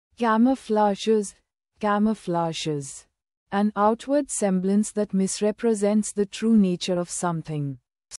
'Camouflages' Meaning and Pronunciation
Word: Camouflages
Meaning: an outward semblance that misrepresents the true nature of something